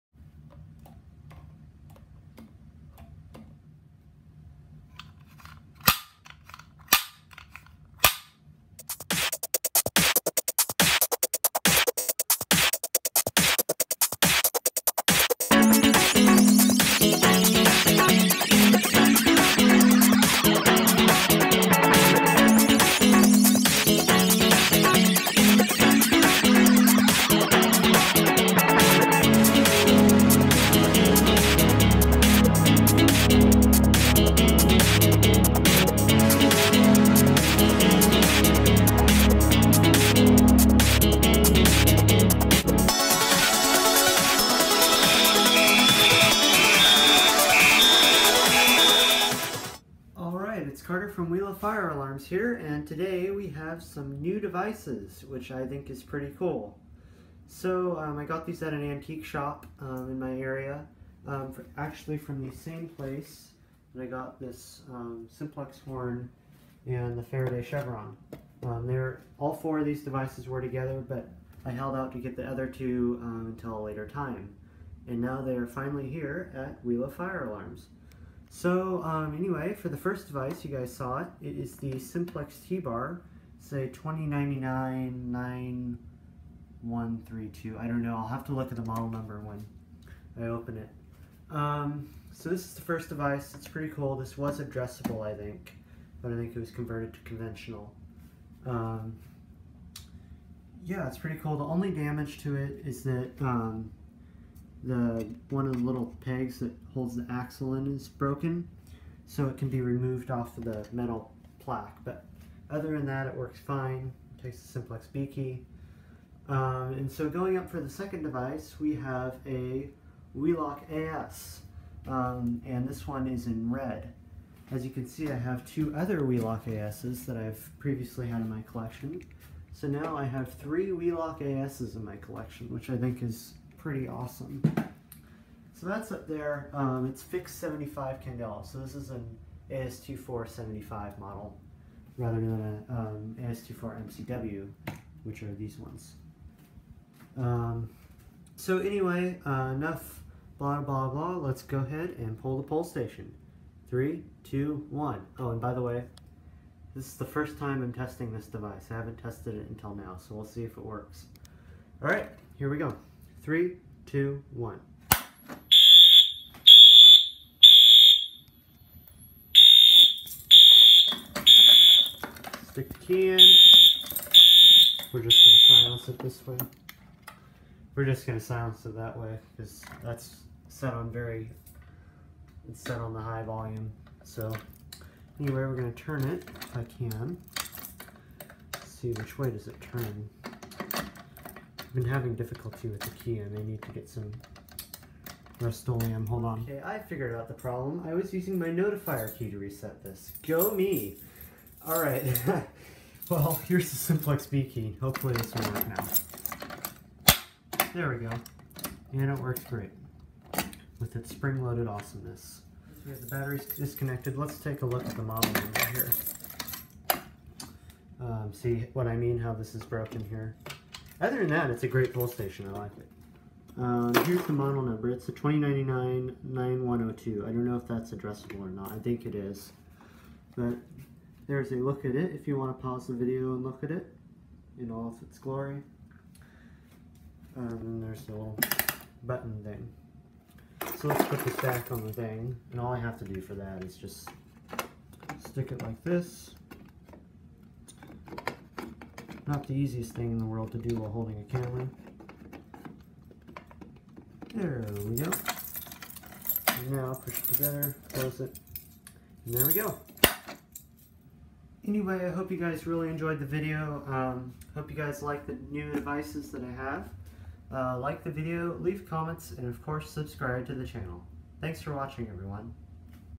More New Stuff!
I found these at the same place that I found my simplex horn and simplex chevron. These are great devices! I can't wait to use them in future videos!